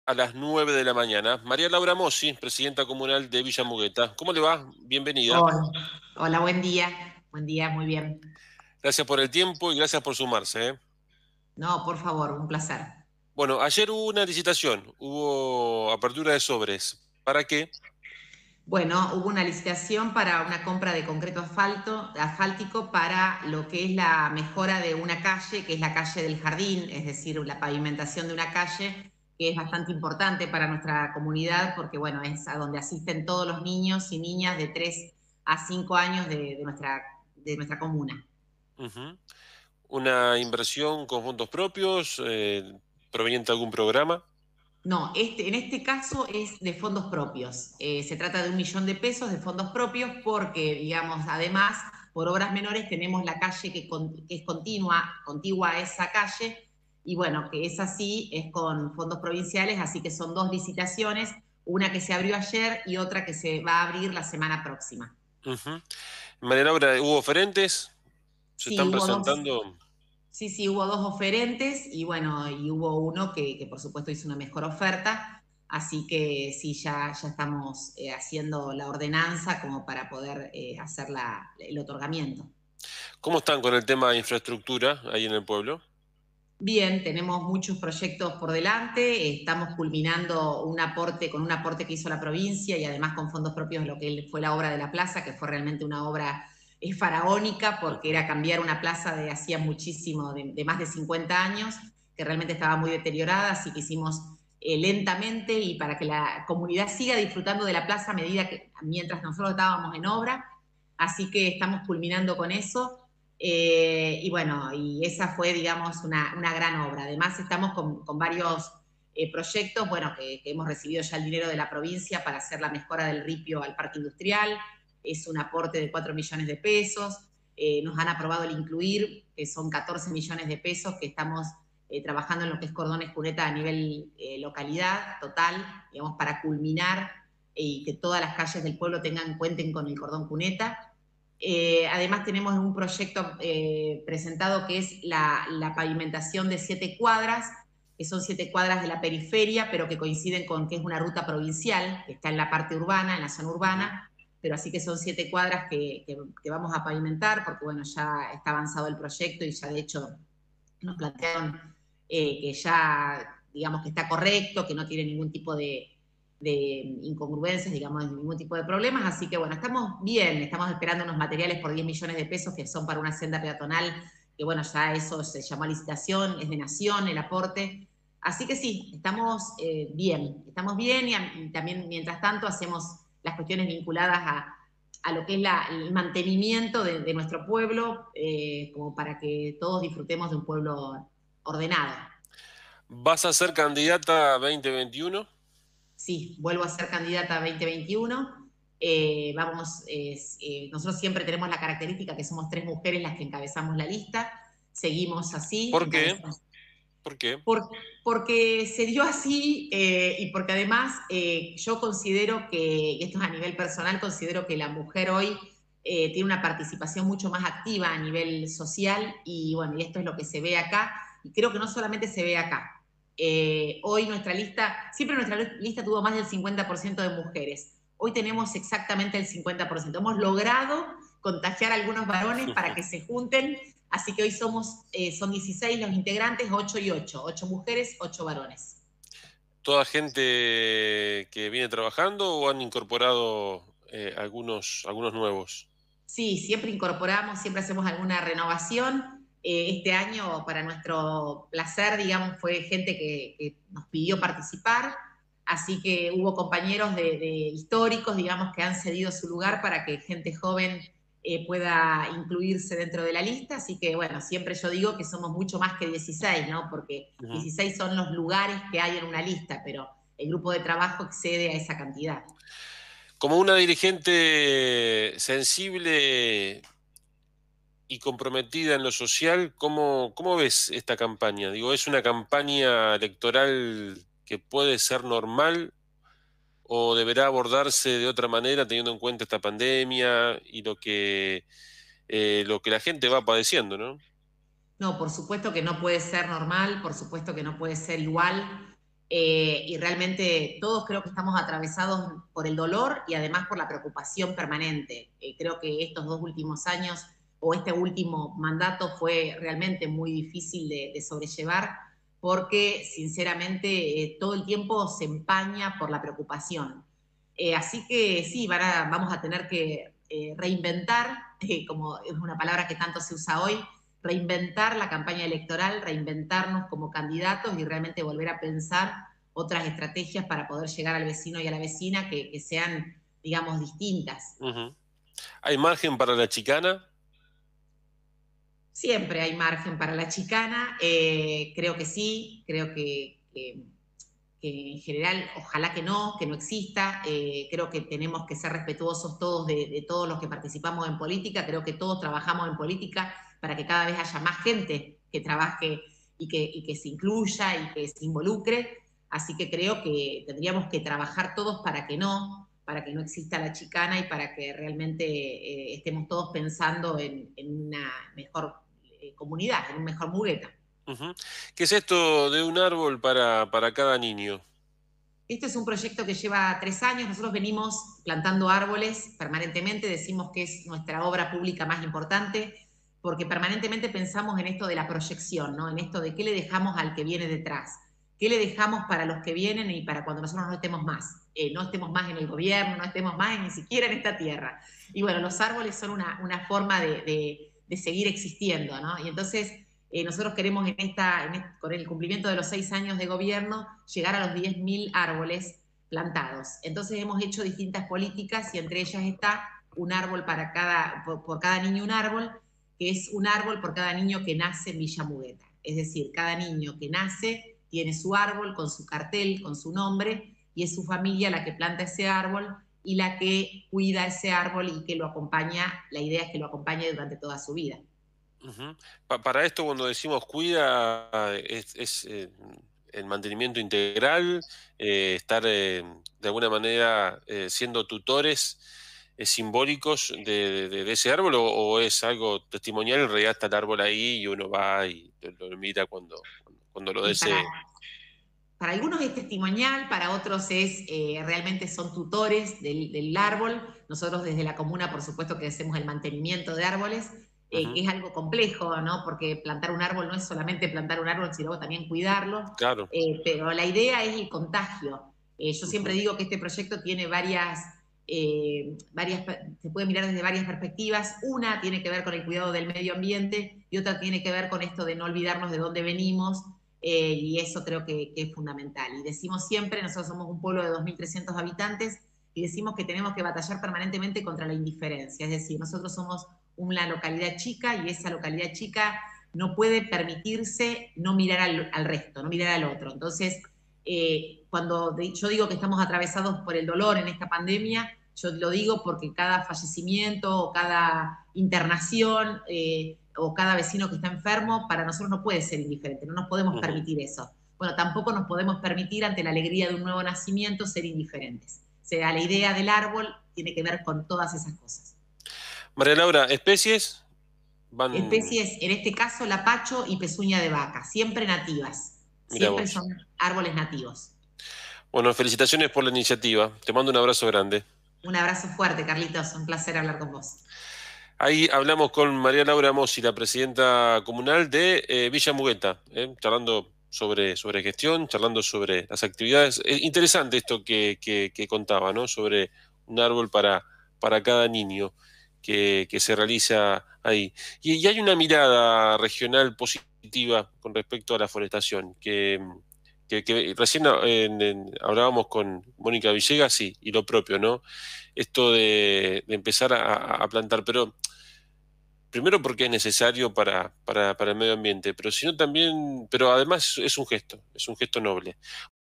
Villa Mugueta invierte en obra pública y va por 10 mil árboles plantados
María Laura Mozzi informó la licitación para la compra de concreto asfáltico que se utilizará para pavimentar la calle del Jardín con fondos propios. Además de confirmar su candidatura a las próximas elecciones, la presidenta comunal repasó las obras en marcha y destacó la importancia del proyecto “Un árbol para cada niño”.